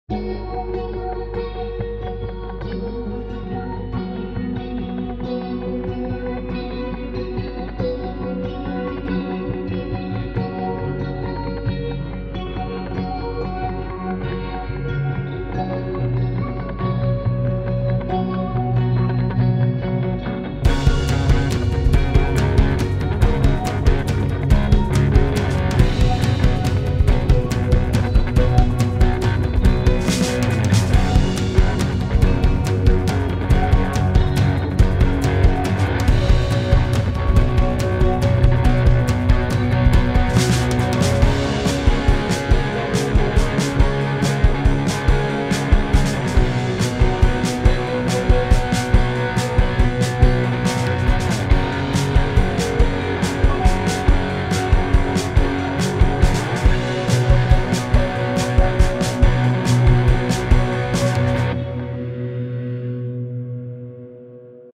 One Minute World- Part 5
The video was captured during my flight back from Bali to Sydney.
The music was recorded by me using garageband.